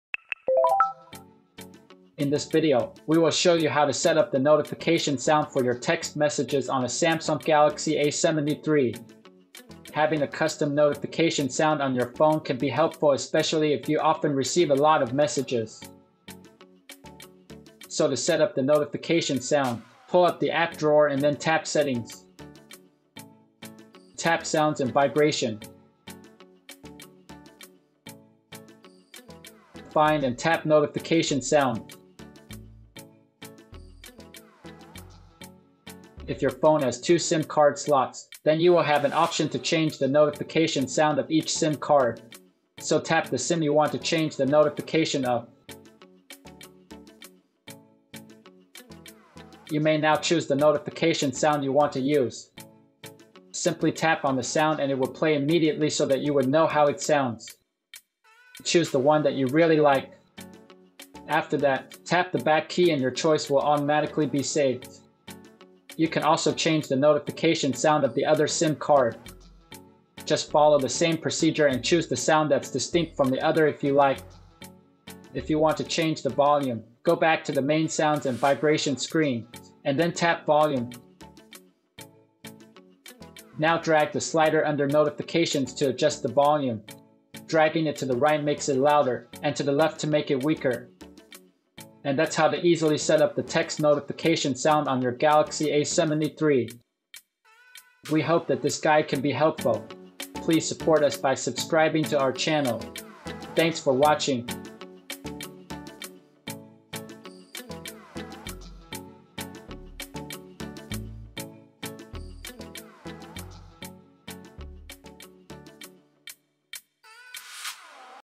How To Set Up Text Notification In Samsung Galaxy A73
In this video, we will show you how to set up the notification sound for your text messages on a Samsung Galaxy A73.

Having a custom notification sound on your phone can be helpful especially if you often receive a lot of messages.

So to set up the notification sound, pull up the app drawer and then tap Settings.

Tap Sounds and vibration.